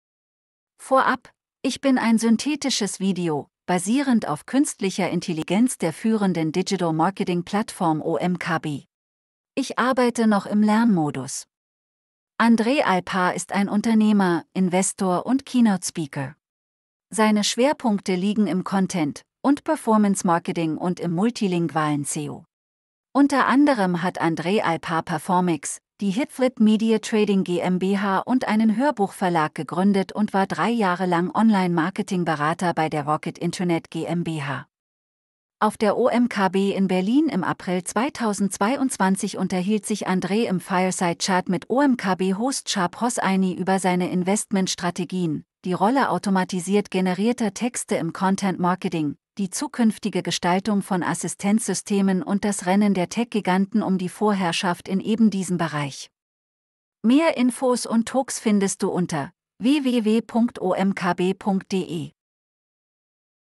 Wer ist Andre Alpar?
Vorab: Ich bin ein synthetisches Video, basierend auf künstlicher Intelligenz der führenden Digital Marketing Plattform OMKB. Ich arbeite noch im Lernmodus.

Andre Alpar ist ein Unternehmer, Investor und Keynote-Speaker. Seine Schwerpunkte liegen im Content- und Performance-Marketing und im multilingualen SEO. Unter anderem hat Andre Alpar Performics, die Hitflip Media Trading GmbH und einen Hörbuchverlag gegründet und war drei Jahre lang Online-Marketing-Berater bei der Rocket Internet GmbH.

Auf der OMKB in Berlin im April 2022 unterhielt sich Andre im Fireside Chat mit OMKB-Host Schahab Hosseiny über seine Investment-Strategien, die Rolle automatisiert generierter Texte im Content-Marketing, die zukünftige Gestaltung von Assistenzsystemen und das Rennen der Tech-Giganten um die Vorherrschaft in ebendiesem Bereich.